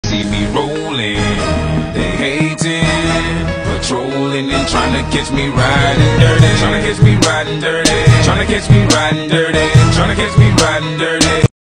Lifted custom golf carts at Ocean Lakes Family Campground during Halloween 2022
Custom golf carts